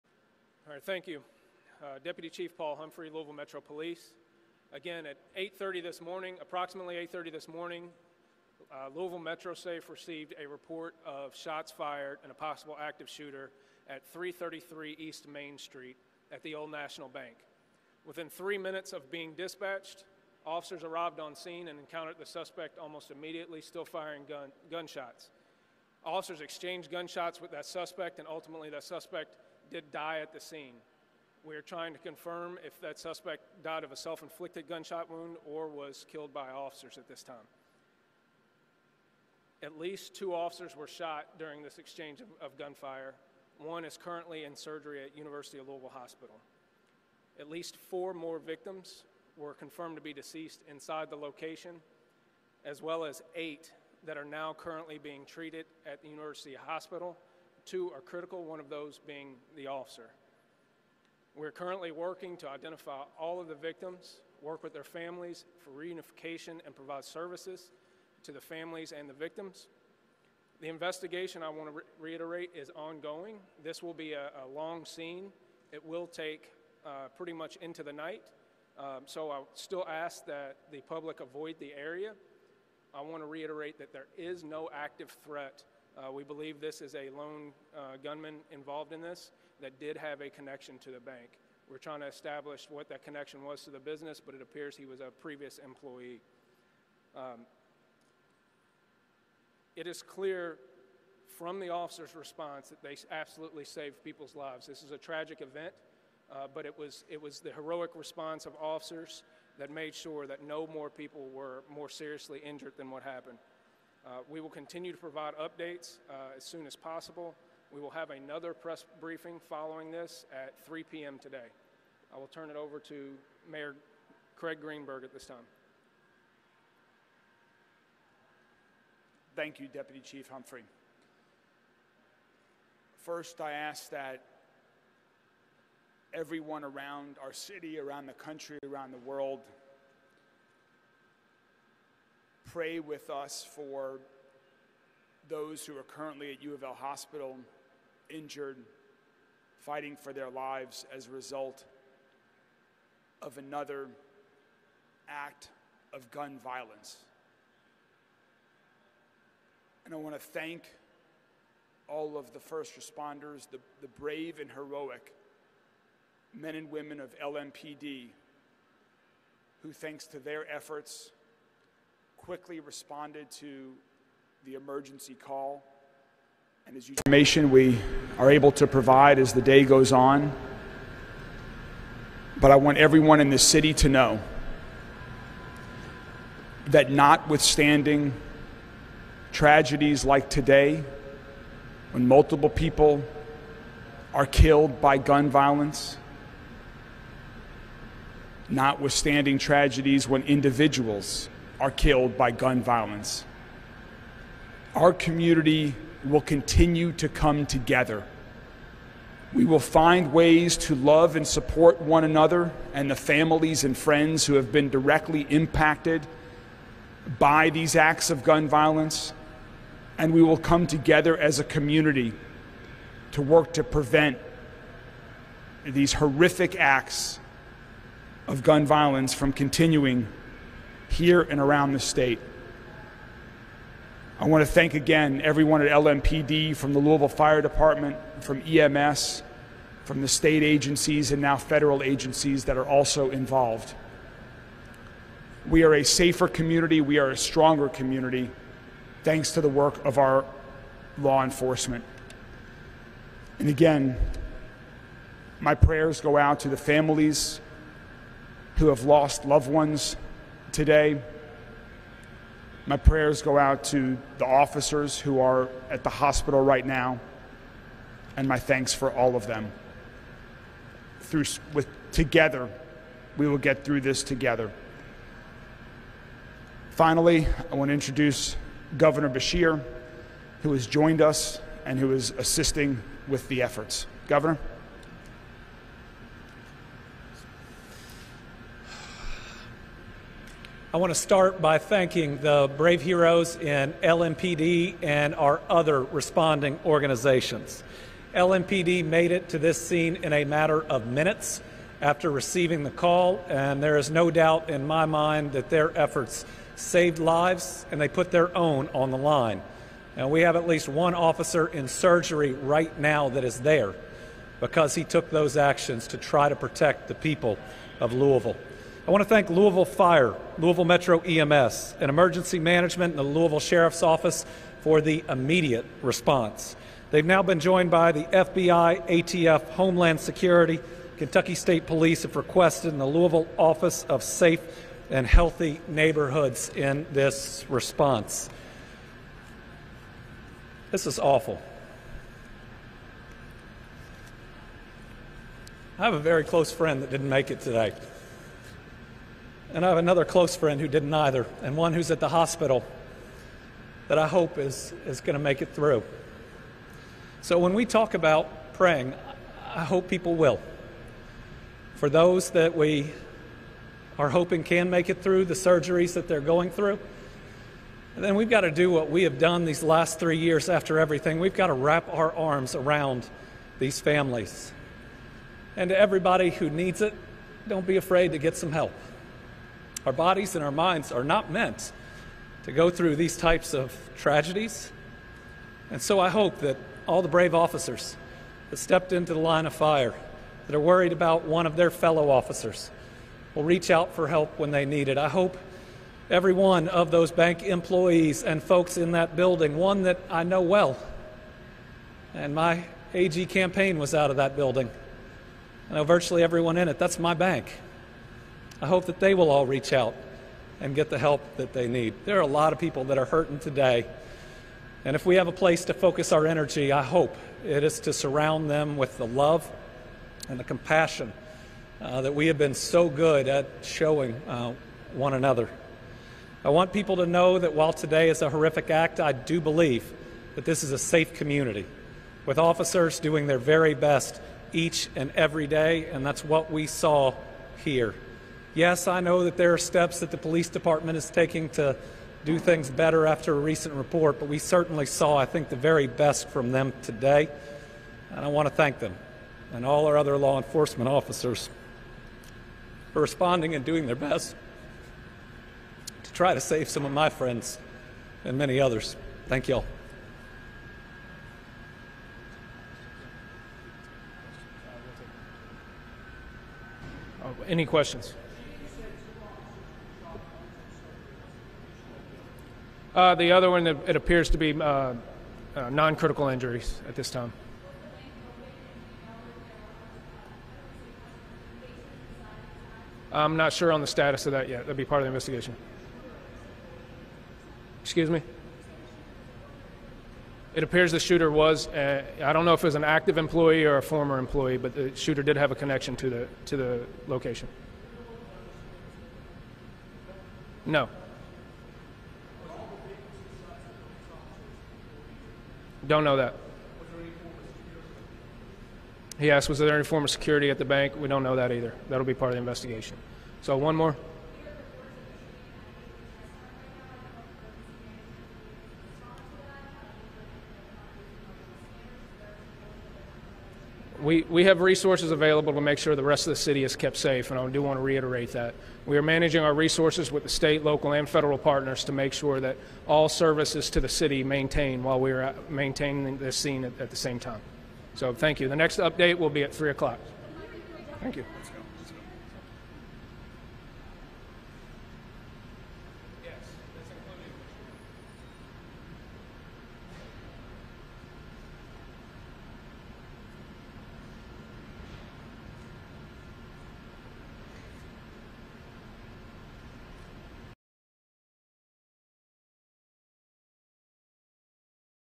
Louisville shooting: 5 dead after suspected employee opens fire in bank
Police and officials in Louisville, Ky., provided an update on Monday after at least four people were killed in a shooting at a bank building downtown. The shooter is also dead after police exchanged gunshots when responding to the incident.

Eight others were brought to the hospital, including one police officer in critical condition, after the Monday morning shooting at the Old National Bank.

Police believe the suspected gunman acted alone and had a connection to the bank, appearing to be a former or current employee.

Kentucky Gov. Andy Beshear grew emotional as he said that he knew two of the people killed and one injured in the shooting.

*Another victim has succumbed to their injuries, increasing the death toll to 6.